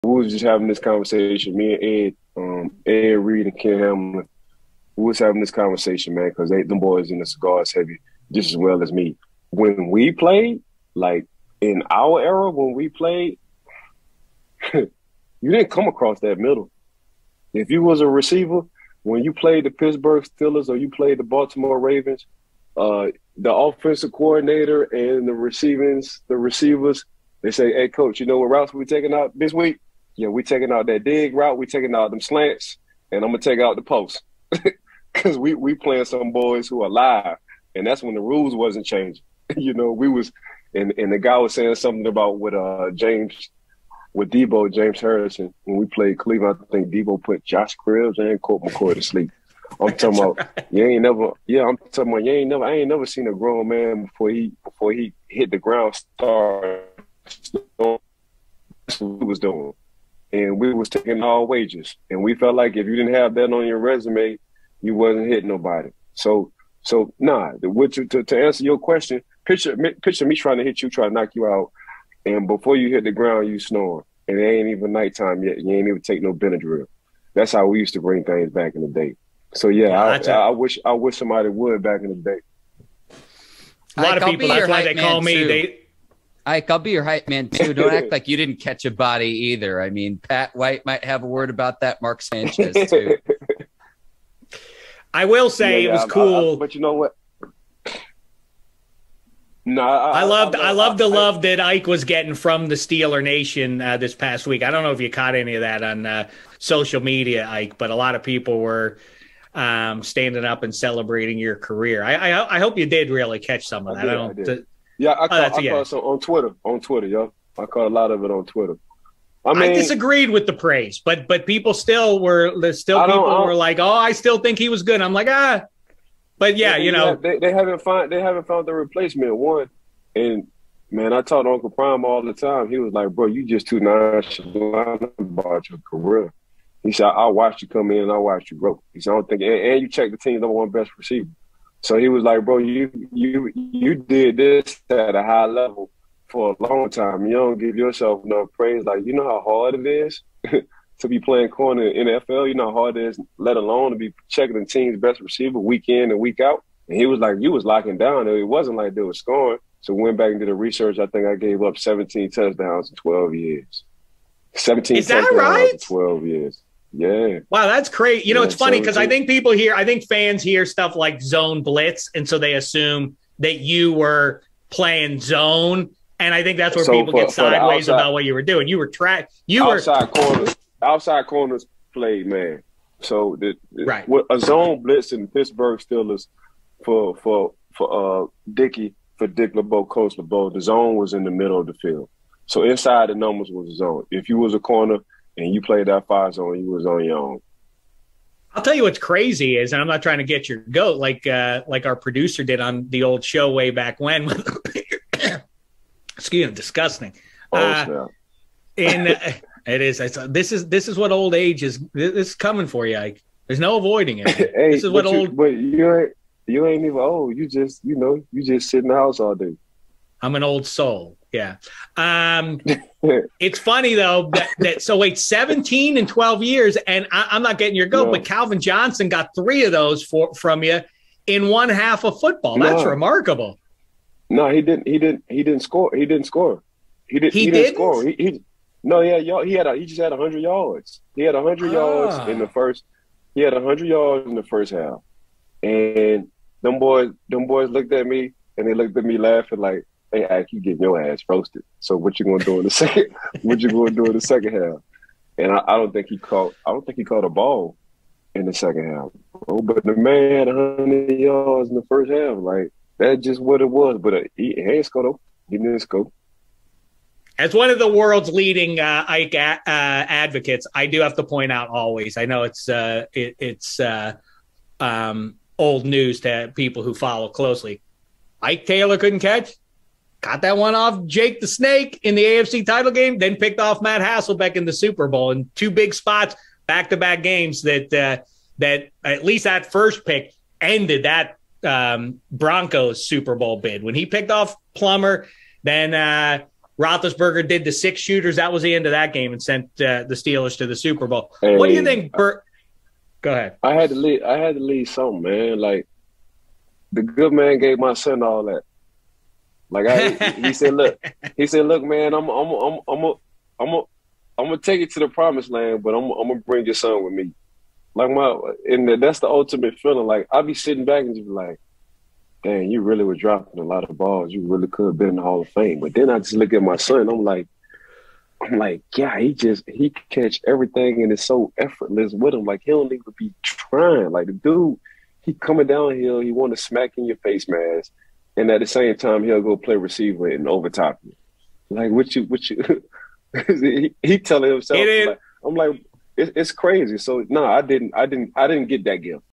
Ike Taylor recalls when James Harrison knocked out Colt McCoy and Josh Cribbs
Former Pittsburgh Steelers cornerback Ike Taylor recalled when his teammate James Harrison knocked out Cleveland Browns quarterback Colt McCoy and wide receiver Josh Cribbs.

Taylor recalled how wide receivers wouldn't go over the middle in his era for fear of getting hit.